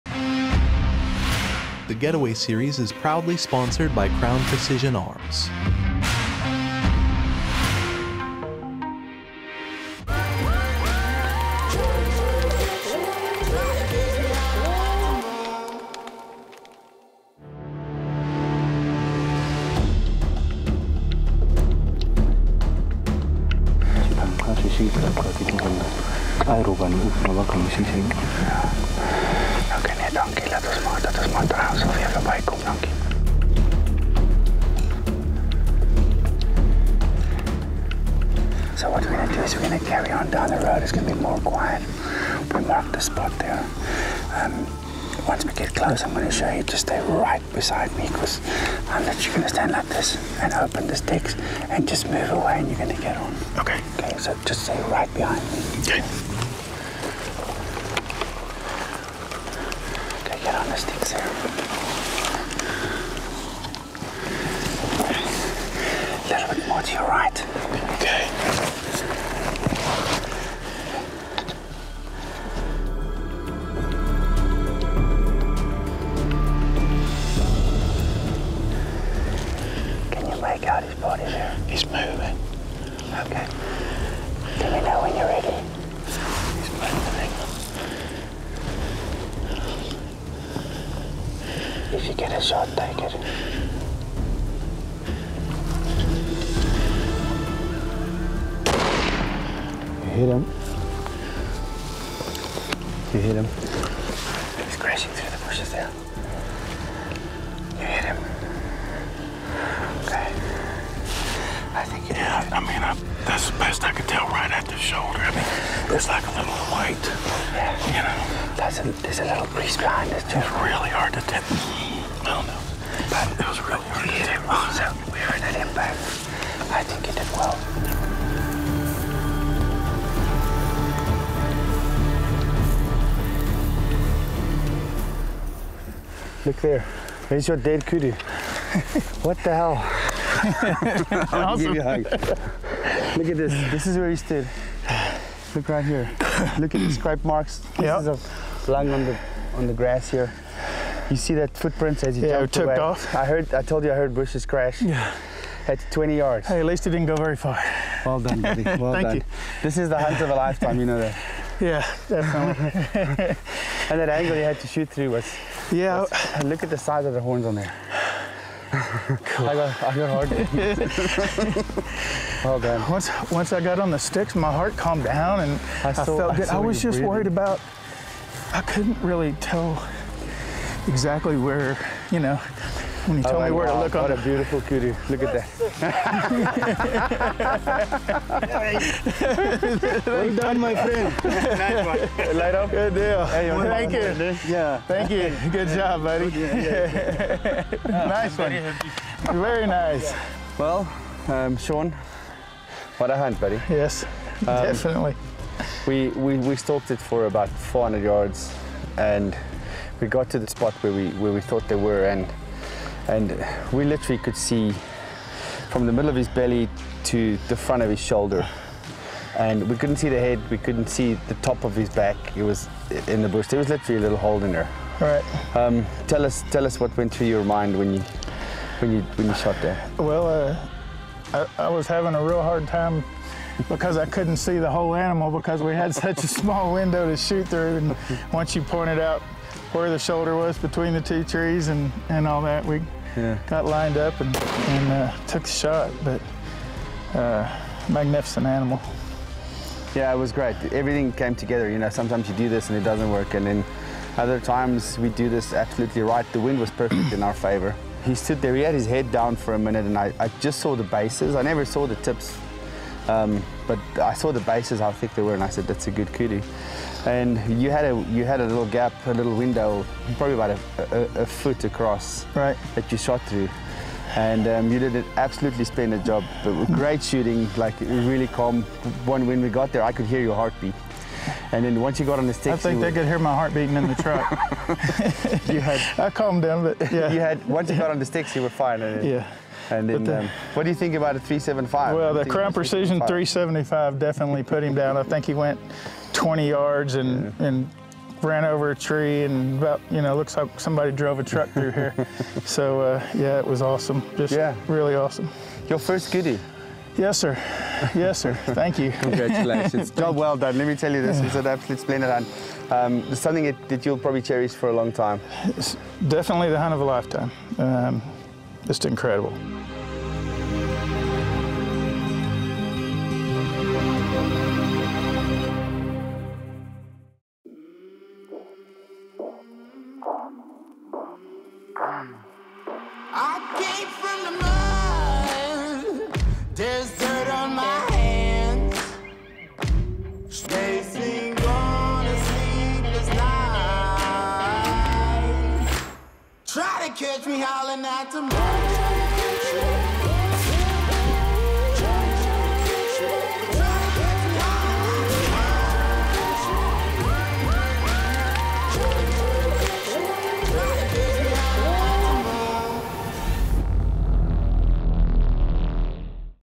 Kudu Hunt In The Thick Bushveld Of Namibia.
Shawn wanted a Kudu. It was his number 1 on his list. Success is never guaranteed with Kudu hunting, but we sure give it our best.